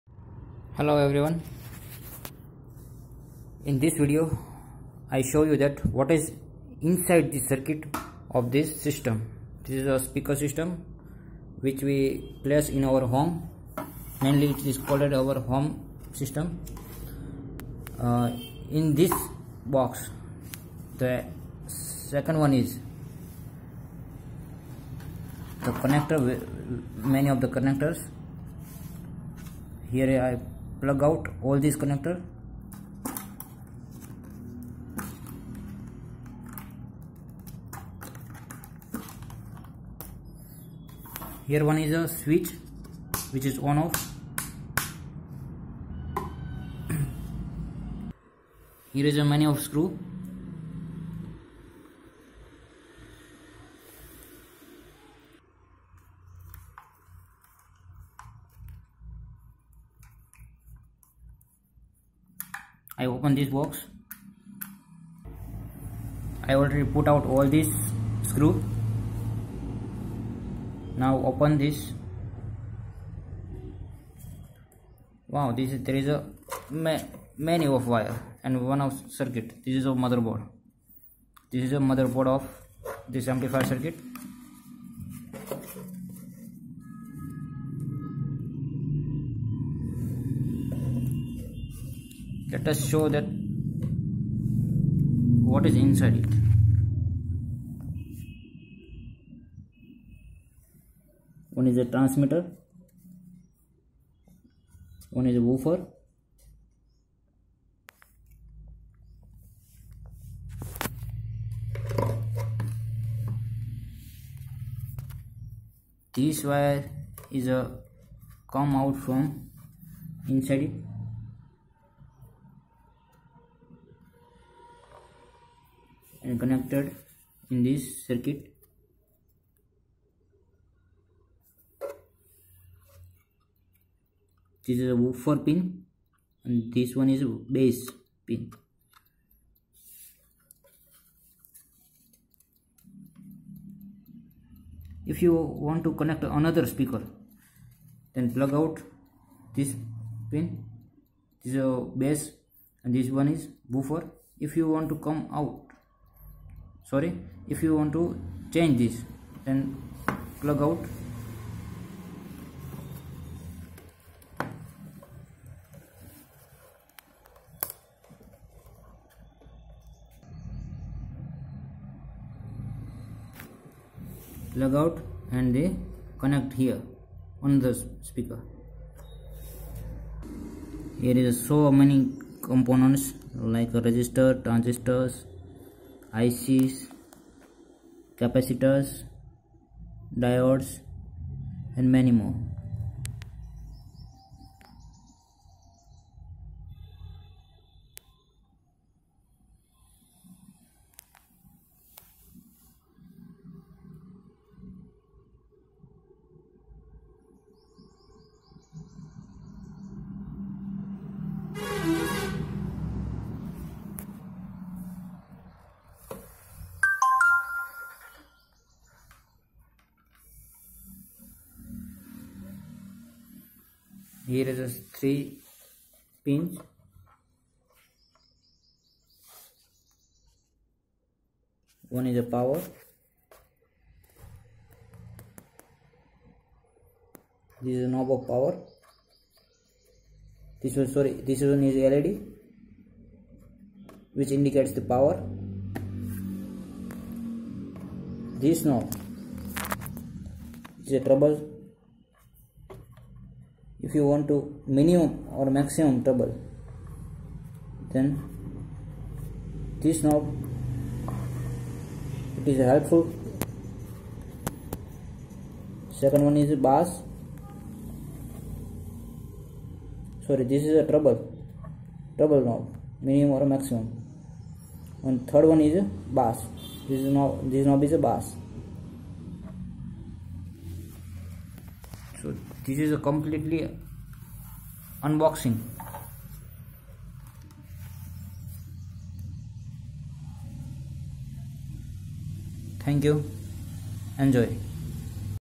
INTEX 2.0 SYSTEM
THIS VIDEO FOE ELECTRONIC LOVERS WHO ARE INTERESTED IN CIRCUIT BOARD AND OTHER MICRO PARTS ARE MANAGED. IN THIS VIDEO I SHOWS YOU WHAT IS INSIDE THE CIRCUIT IN INTEX 2.0 AUDIO SYSTEM.